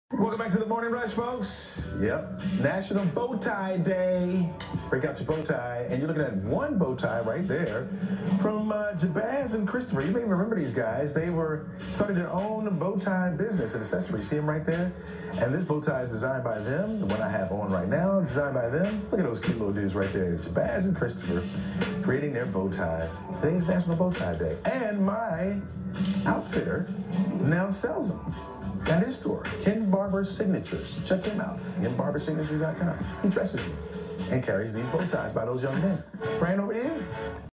National Bow Tie Day 2018
Chesley McNeil wears Kings and Gents Accessories to celebrate National Bow Tie Day!!

Backdrop Lighting

Ring Light

Green Backdrop

Adjustable Backdrop Support Stand

Lapel Microphone

Cannon Rebel

Phone Tripod

Tripod with Remote